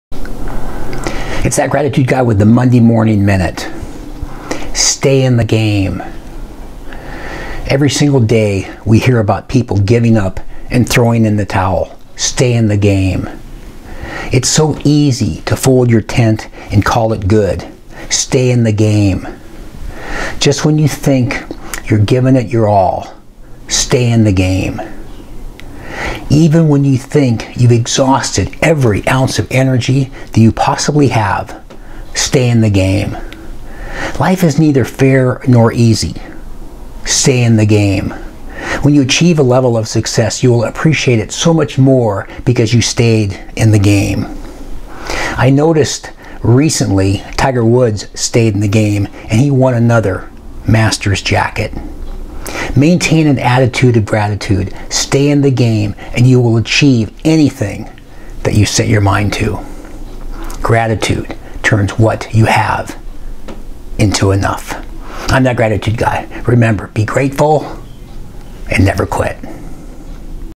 Stay in the Game
Every single day we hear about people giving up and throwing in the towel…. Stay in the game.

It is so easy to fold your tent and call it good…..Stay in the game.

Just when you think you’ve given it your all…..Stay in the game.

Even when you think you’ve exhausted every ounce of energy that you have…..Stay in the game.

Life is neither fair nor easy…. Stay in the game.

When you achieve a level of success you will appreciate it so much more because you stayed in the game.

I noticed recently Tiger Woods stayed in the game, and he won another Masters jacket.

Maintain an attitude of gratitude, stay in the game, and you will achieve anything that you set your mind to.